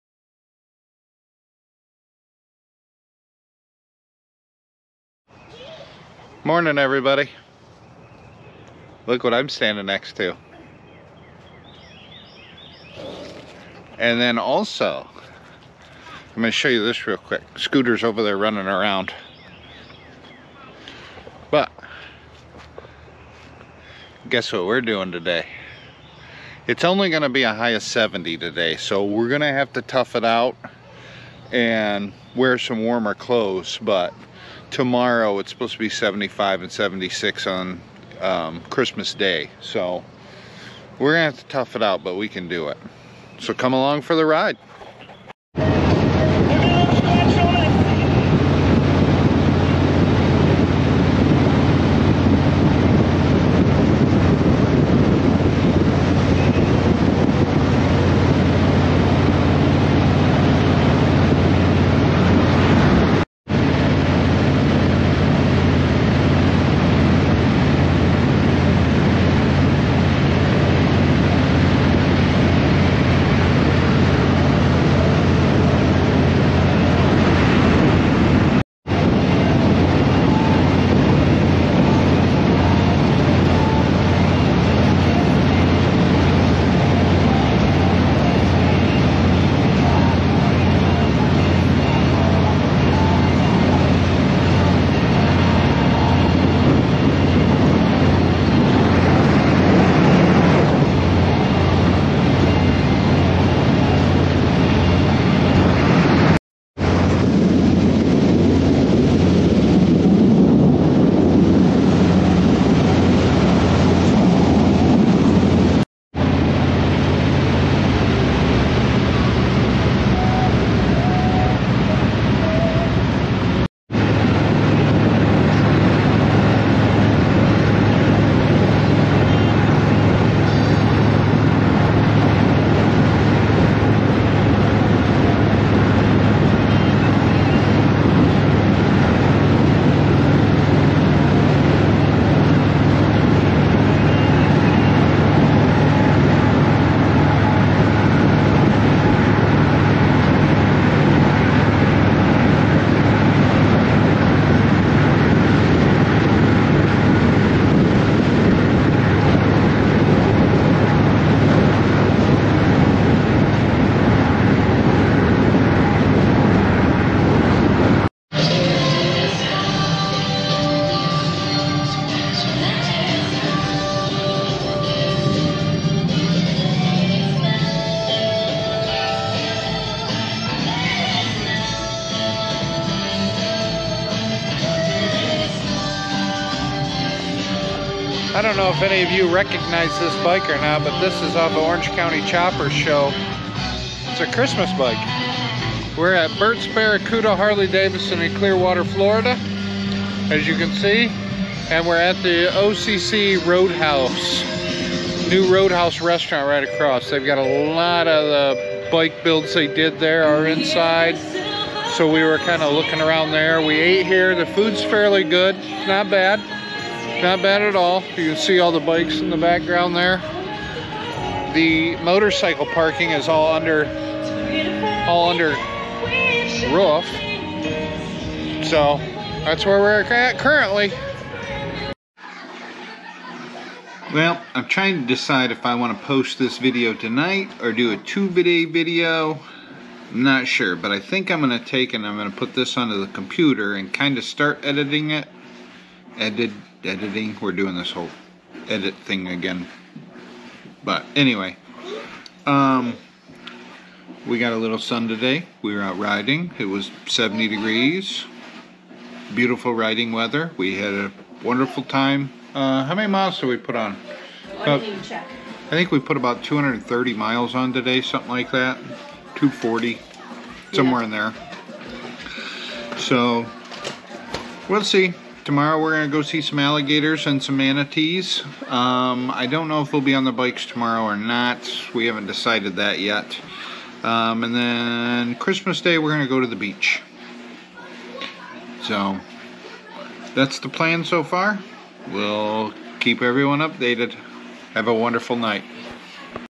quick little ride before Xmas in Florida
The day started out at 47 degrees, but ended at 71 degrees.